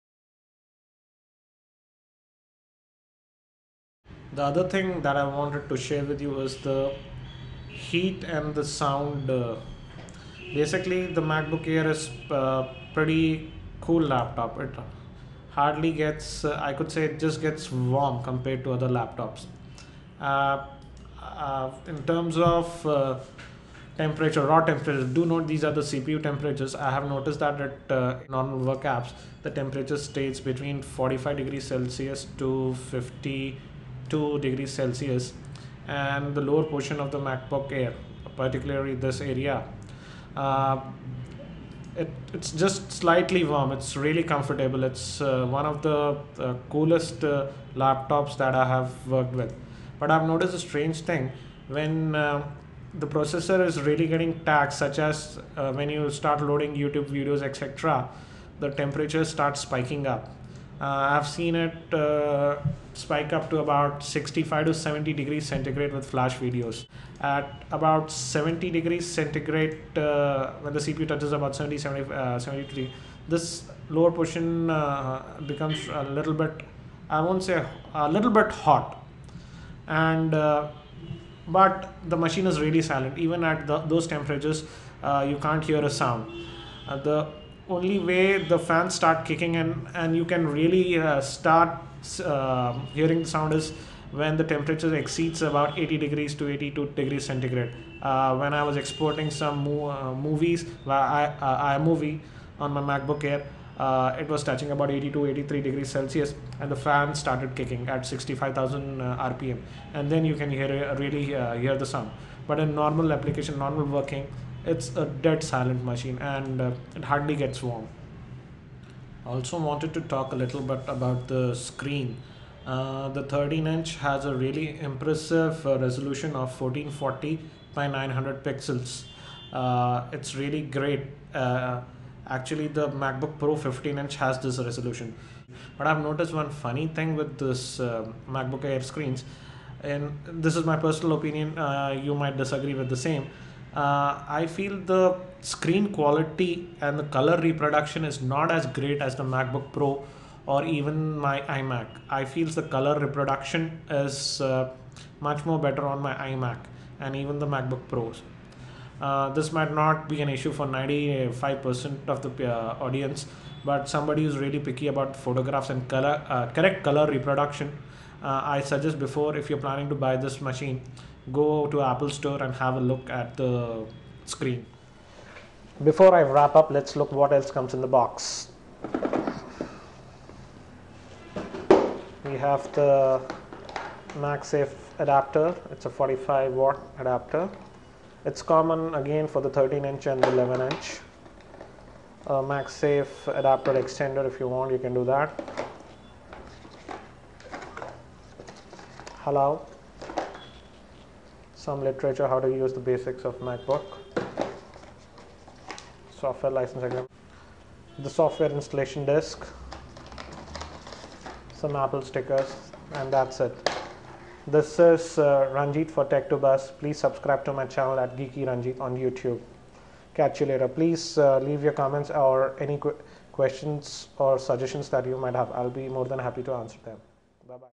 13" 2010 base Macbook Air Review Part 2
Review of 13" 2010 base Macbook Air after using it for 45 days Part 2